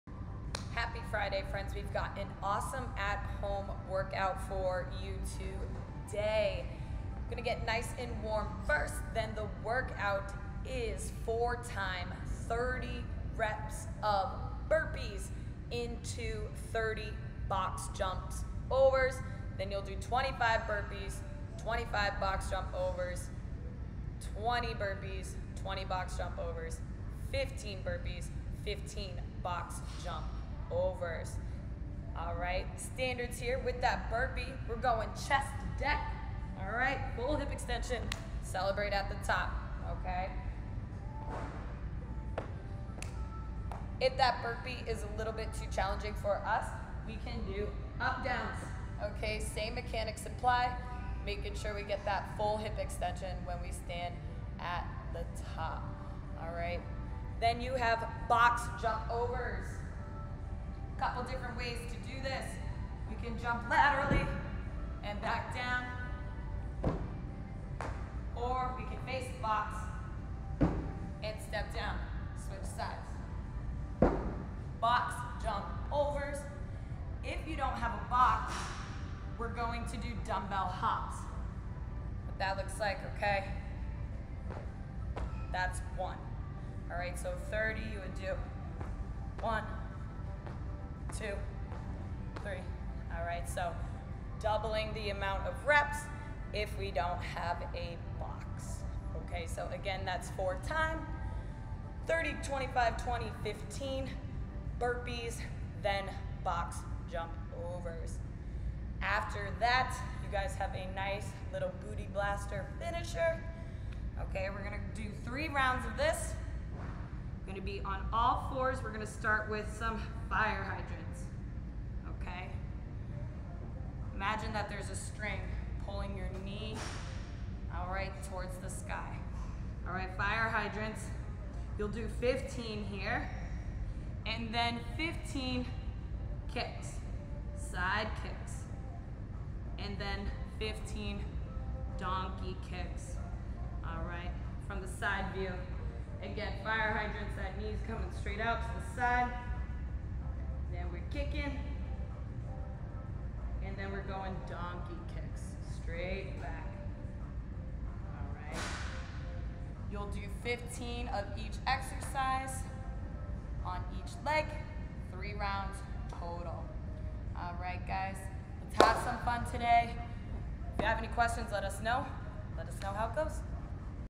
Home Workout Friday 10 16 2020
For Time:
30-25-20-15
Burpees
Box Jump Overs